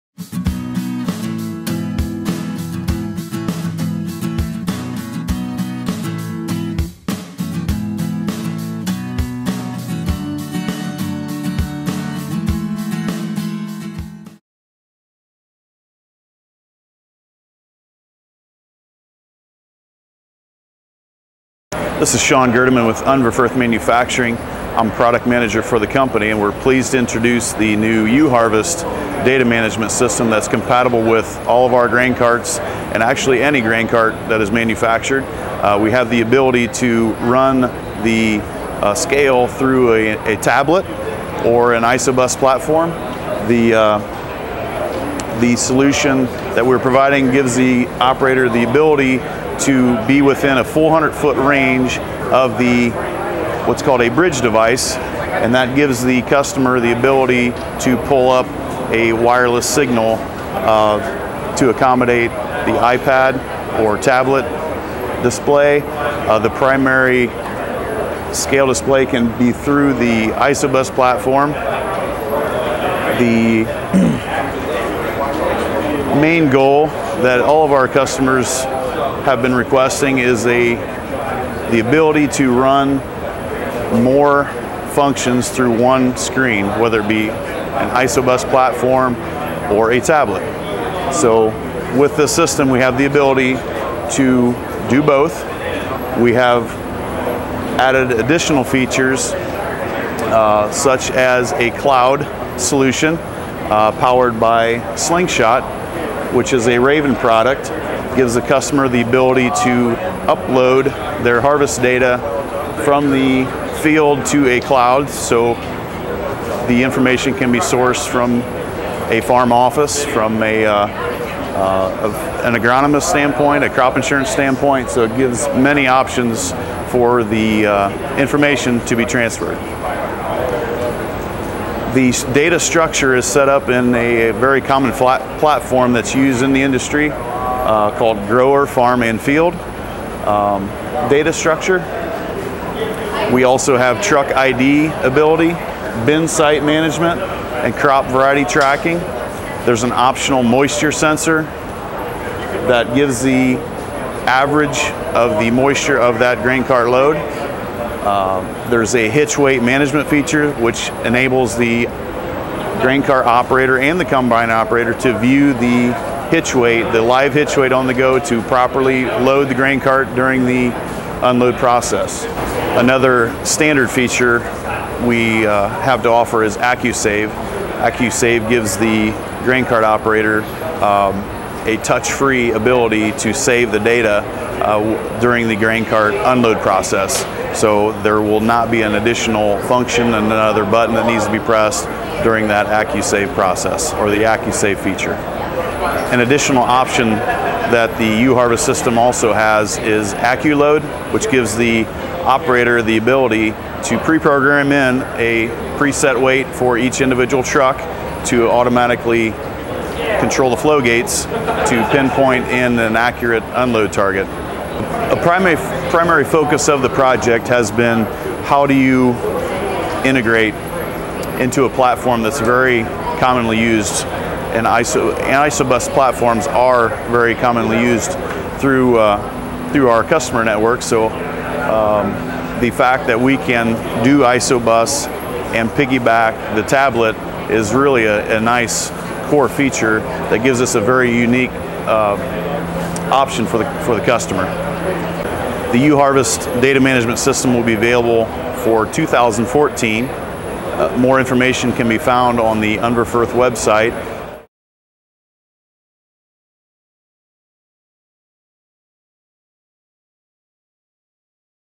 Unverferth Introduces UHarvest Data Management Solution
Unverferth Manufacturing Company, Inc., announces the introduction of the UHarvest data management system, the first-ever solution for collecting and managing grain cart weight data using the tractor's ISOBUS terminal or a touch-screen tablet.

Grain carts equipped with load cells are the traditional way to accumulate harvest data.  The Unverferth UHarvest data management system, powered by Raven Industries, combines software that allows the user to easily track and analyze as much harvest data as needed with state-of-the-art touchscreen operation.

The touch-screen interface offers a quick and convenient way to track the standard grower, farm and field information as well as crop, variety, truck identification and destination of every bushel that moves through the grain cart. The UHarvest system's AccuSave technology also features a drivetrain sensor attached to the cart for automatically recording each load for data accuracy without the need for operator intervention.

The UHarvest system offers easy installation on any grain cart equipped with scales. Its ECU attaches to the grain cart and connects to the scale load-cells. The processor plugs into the existing tractor ISOBUS connection and is ready to operate.

For tablet operation, the UHarvest processor's built-in WiFi signal provides a reliable and secure connection. This closed-system Wi-Fi also provides connectivity for allowing others within a 400 foot radius of the processor to view the grain cart's information via password-protected access using a tablet computer or smartphone.

The UHarvest software also provides a combine verify mode for calibrating the combine's yield monitor in real time and a hitch weight management feature for helping the combine and grain cart drivers keep the cart's contents evenly distributed.

Harvest data can be downloaded from either the processor's USB port to a thumb drive or uploaded with a few touches to a secure Raven Slingshot® web-based server when using a tablet with a data plan.  This securely stored information can then be used for reporting and sharing with landlords, farm managers, agronomic advisors, crop insurance agents or others.

UHarvest system options include AccuLoad for automatically loading a predetermined amount of grain into semis or wagons and Moisture Sense technology for automatically storing and converting each load to dry bushels.

Availability of the new system is slated for 2014.  For additional information, contact your nearest Unverferth UHarvest system dealer or visit Unverferth.com.